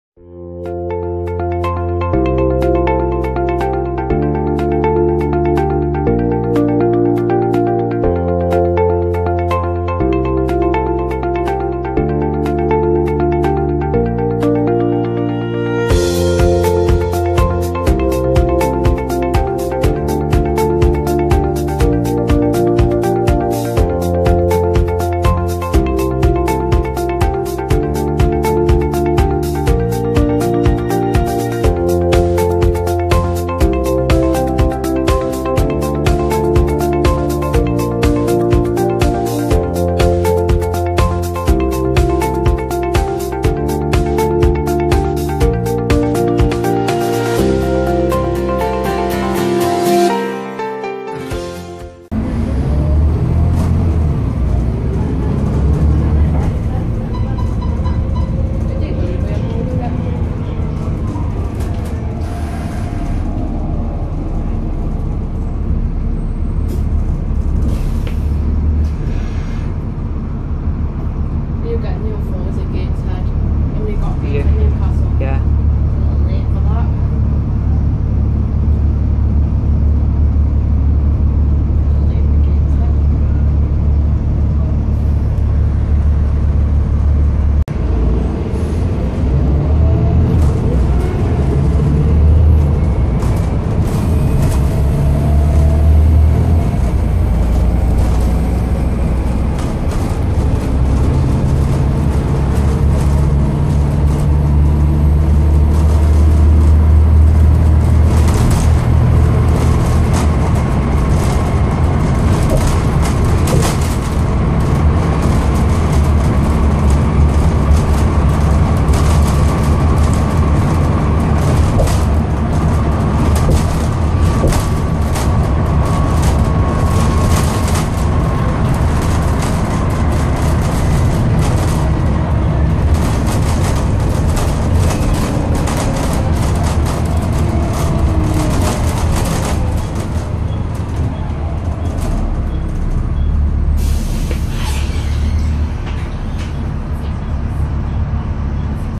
Go North East LX54 GZB/6924-Volvo B7Tl/Plaxton President
Welcome to another video today we are riding Go North East LX54 GZB/6924. Captured here operating service 1 towards North Shields.